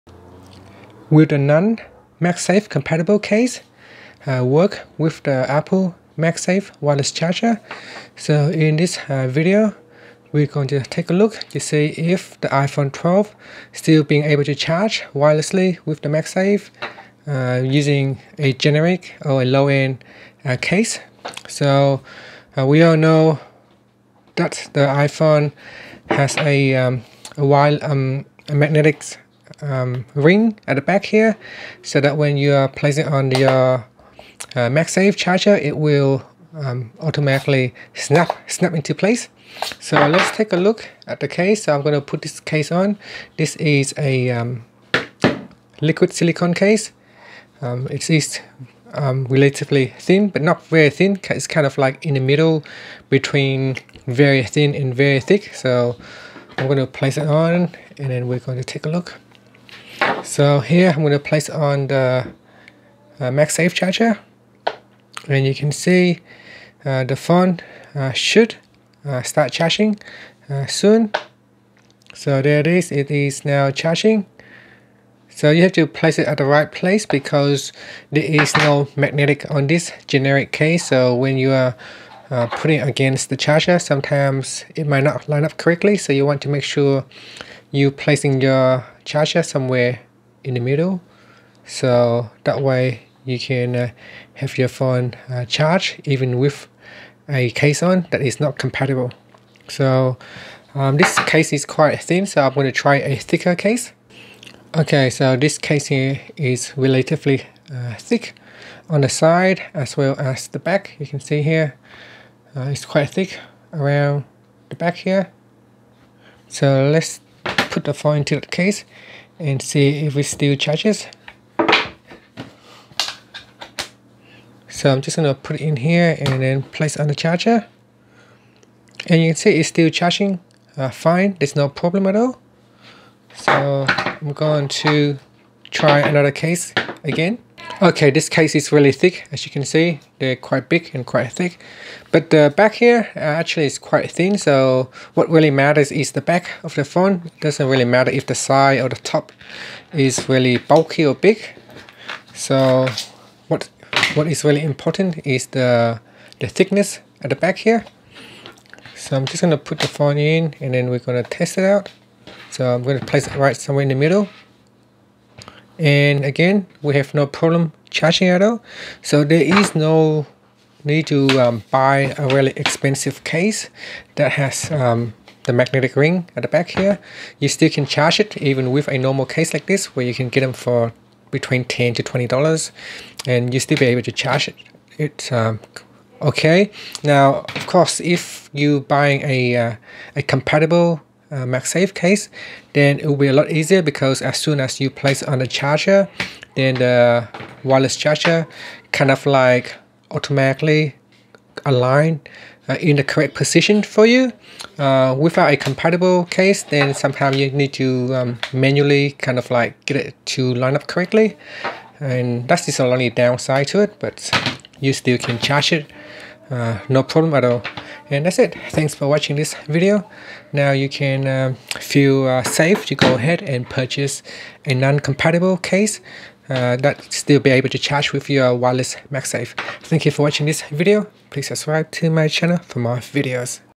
iPhone 12/12 Pro: Testing Non Magsafe Compatible Case With Apple Magsafe Wireless Charger?
See if you can use a non Magsafe compatible case to charge your iPhone 12/12 Pro with a Magsafe wireless charger.